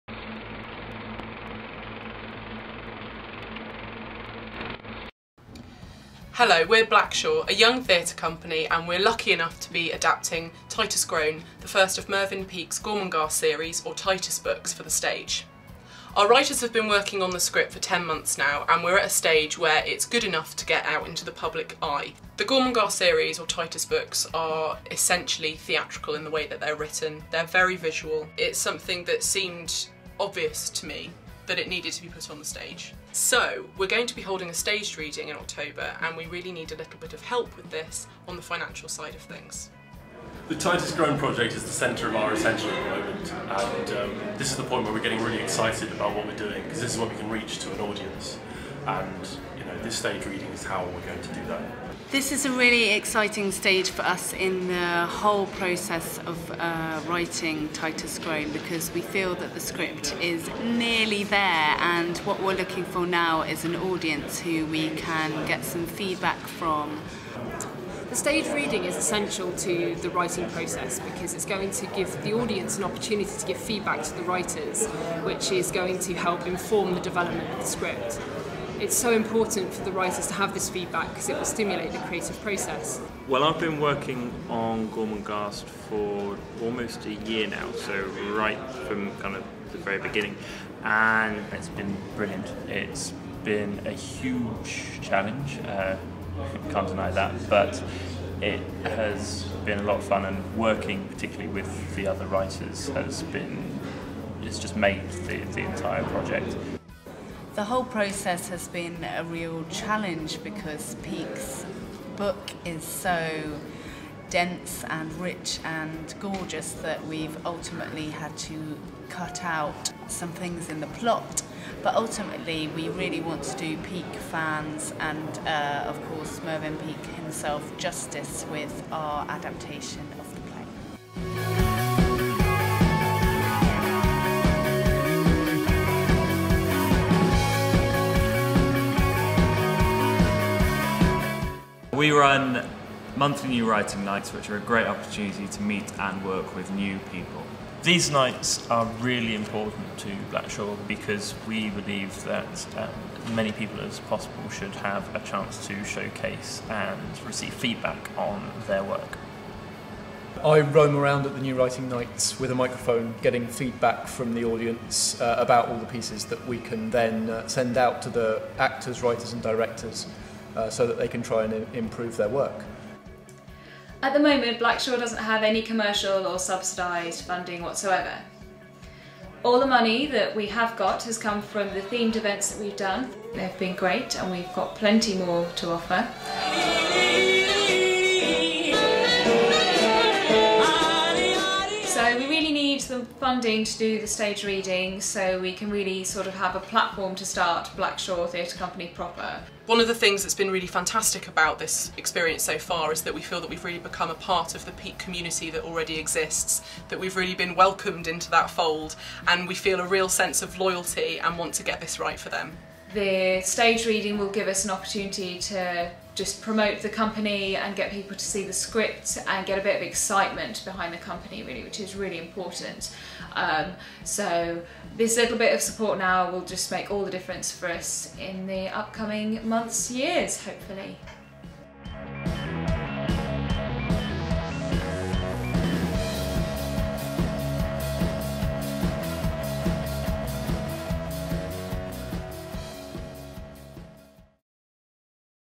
Gormenghast: Titus Groan
In October 2010, Blackshaw secured the contract to adapt the first of Mervyn Peake&rsquo;s classic Gormenghast series, Titus Groan, for the stage.&nbsp; We have been working on the script for almost a year now, and are ready for the first public viewing.&nbsp; This WeFund project will allow us to host a reading of extracts from our script on 6th October 2011.